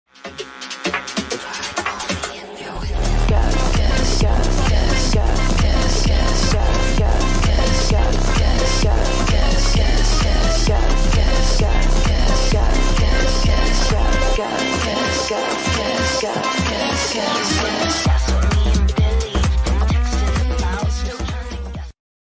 GUESS YCH animation meme {CLOSED}
YCH OPEN Bid
❗️rules❗️
Payment:pay pal
-characters can be human or furry (eg:wolf)
-characters can be any gender
-I will start the meme when I get the complete payment
-I will keep showing updates
-no refunds unless I don’t complete the meme
-bid ends 48 hours after latest bid, 24 hours if AB

Minimum increase: £10
Auto buy 1: £100 (custom thumbnail and cell shading on the animation and drawings)
Auto buy 2:£170 (previous + 4 characters animated instead of 2)
This is only a sketch. The finished product will be fully coloured  🥰